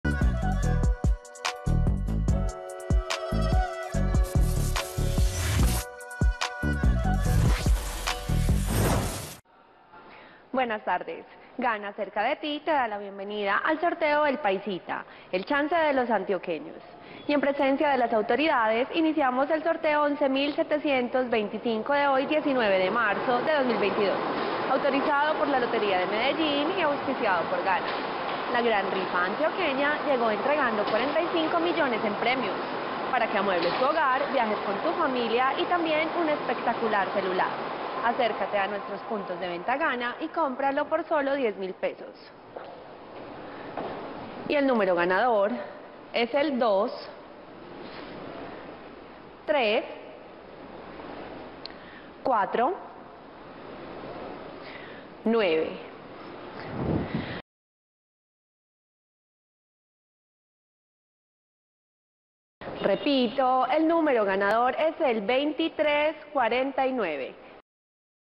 EL PAISITA 2 Resultados del Chance del SÁBADO 19 de marzo de 2022 ✅🥇🔥💰
Hola buenas noches, te presentamos los resultados de las loterías y chances en Colombia, vamos con el Resultados del Chance El Paisita 2 del SÁBADO 19 de marzo de 2022.